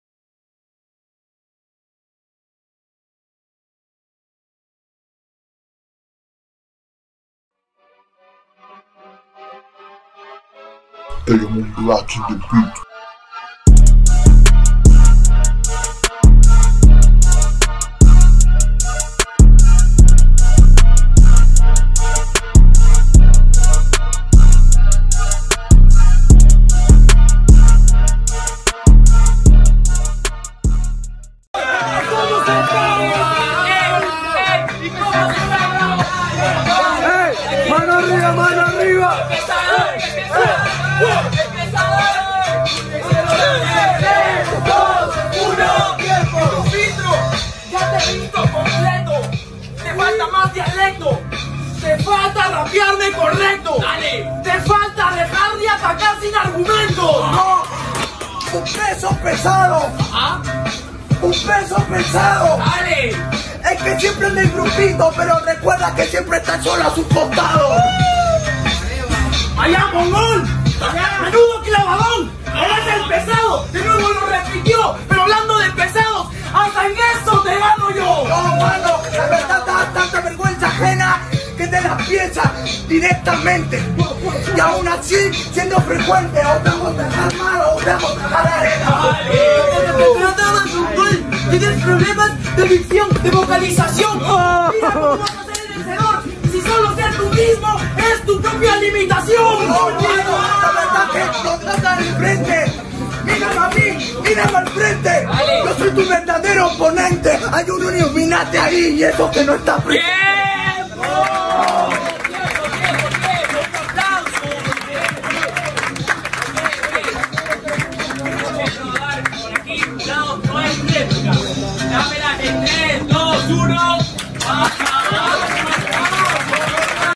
PANCHERO (RPK) VS DARKS (RPT) - OCTAVOS - MAGDALERAP - CRUCE DE COLECTIVOS 2022
Aviso: La fecha esta desactualizada y es de hace dos años. Los freestylers presentes en los videos están retirados o cambiaron de teams. Ya disponible el primer Cruce de Colectivos que se dio entre Magdalerap, Rapnarok y Colectivo Raptors.